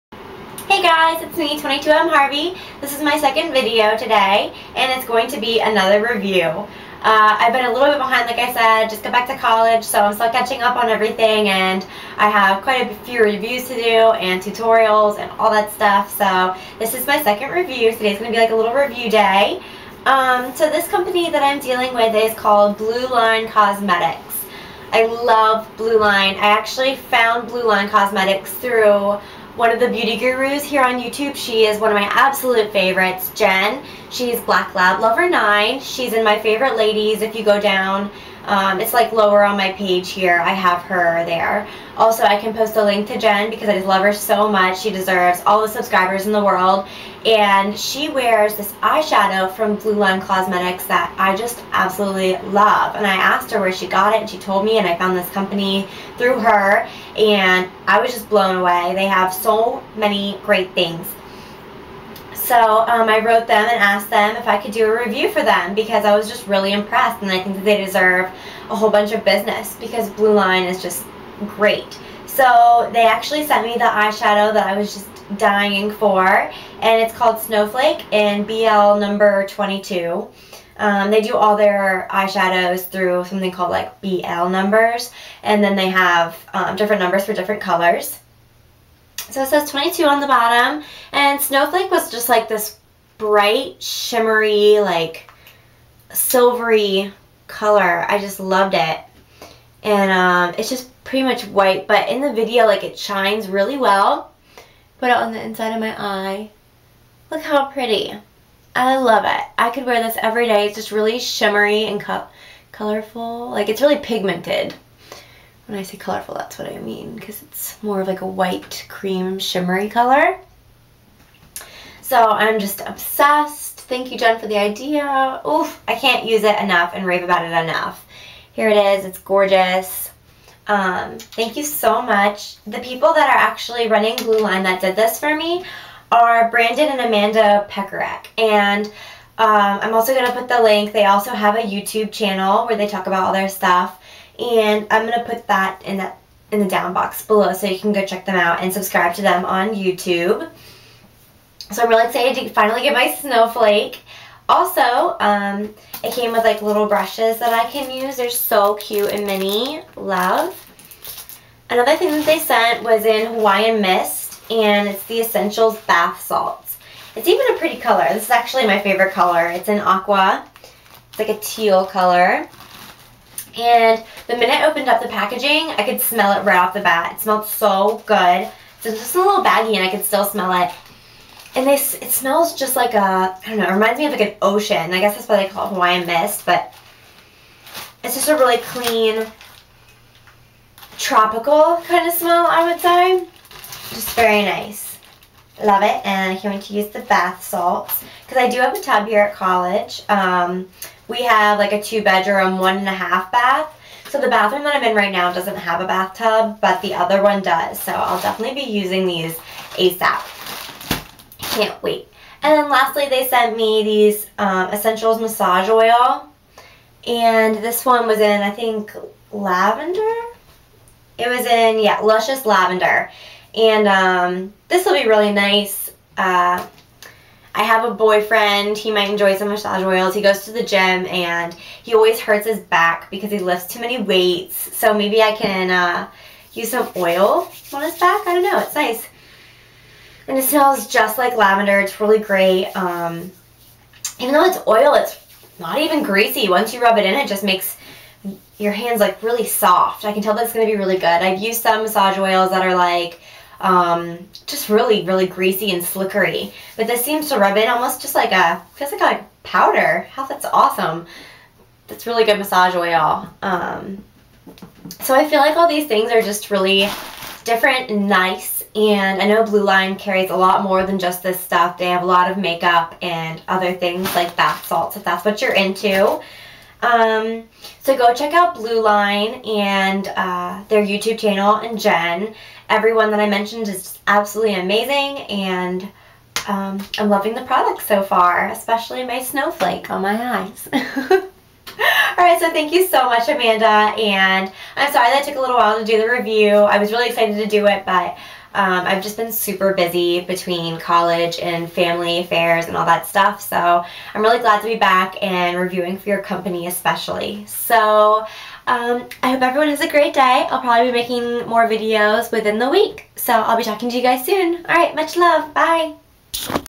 Review: BlueLine Cosmetics
Love love love this company! Here is a review of a few of their amazing products!!! Visit their website and YT channel:

Found them through BlackLabLover9 :)